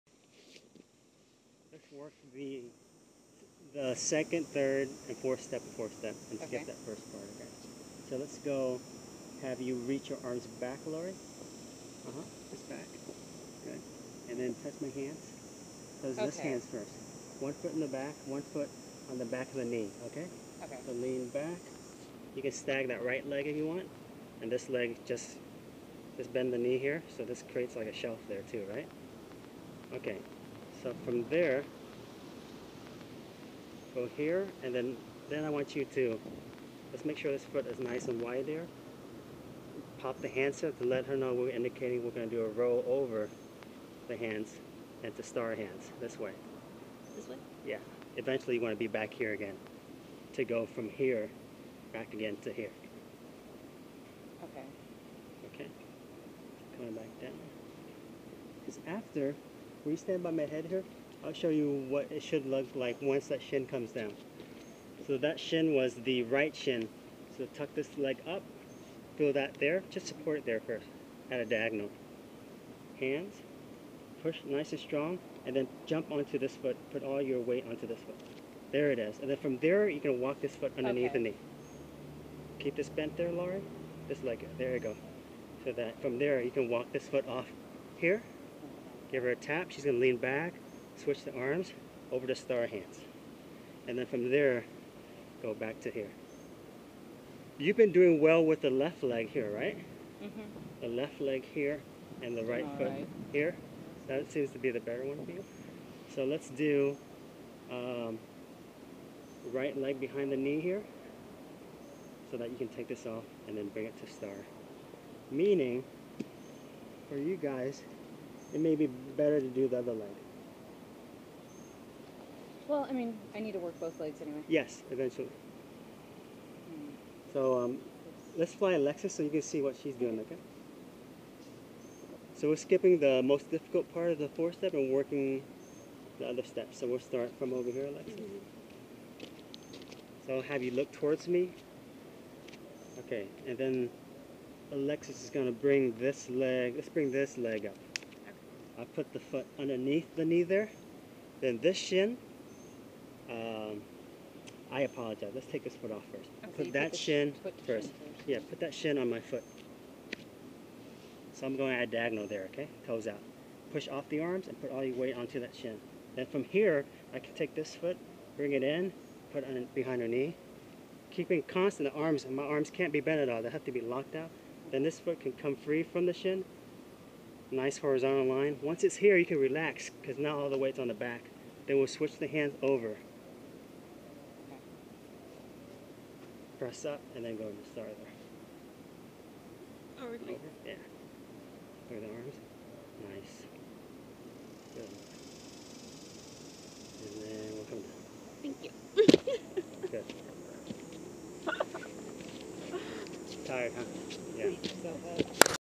Four Step, Ninja Star Four Step Part 1 - AcroYoga Washing Machine Tutorial
This video demonstrates some helpful tips to get to started with Four Step.